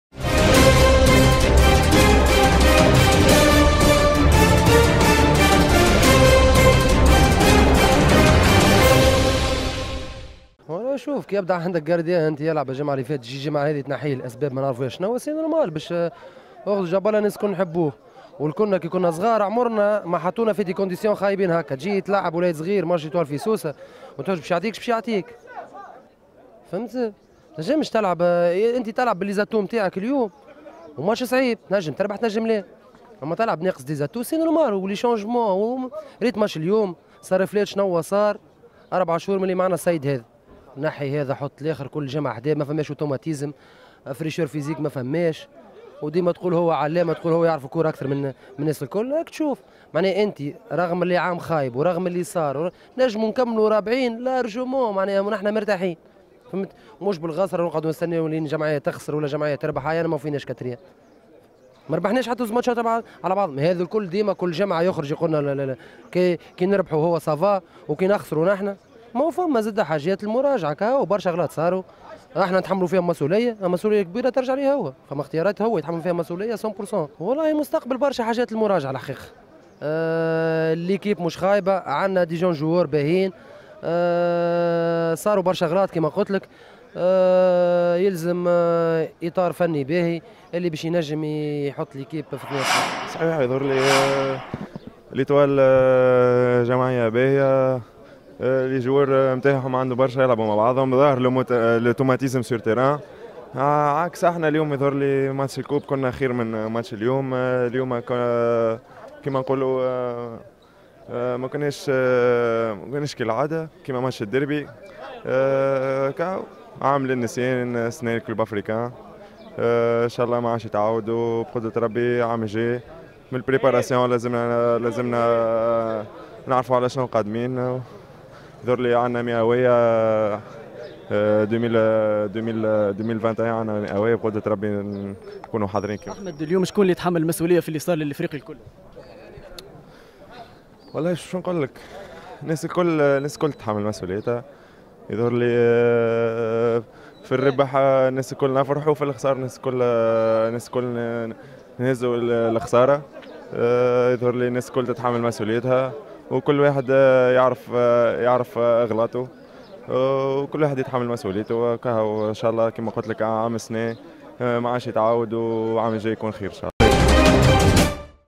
زهير الذوادي و أحمد خليل يتحدثان عن موسم النادي الإفريقي
تبّع أخبار الكوورة التونسية و العالمية على فوت 24